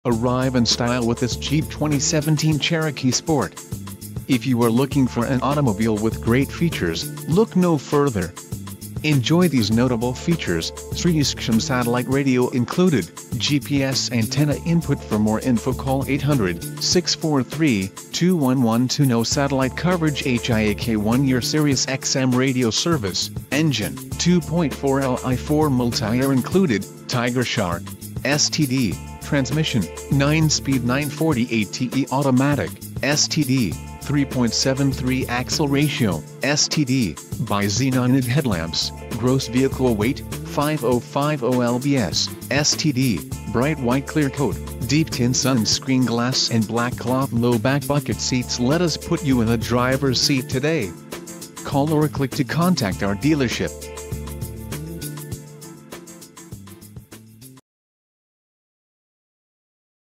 2017 Jeep Cherokee Sport
2017 Jeep Cherokee Sport

Condition: Used
Mileage: 17033
Transmission: 9-Speed Automatic w/OD
Exterior Color: White
Interior Color: Copper
Doors: 4
Stock
Cylinders: 4